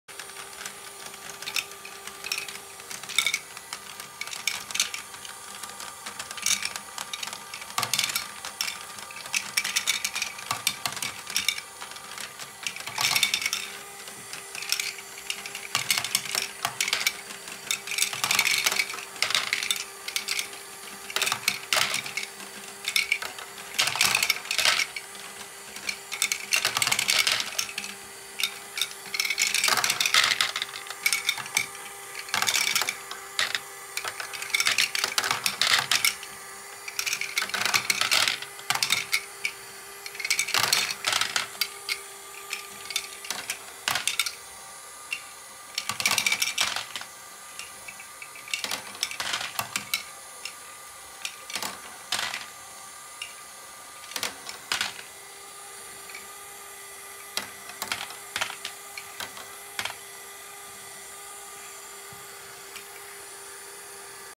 Lego GBC Module - Double Bucket Wheel
Here is my version of Berthil van Beek's Double Bucket Wheel GBC module.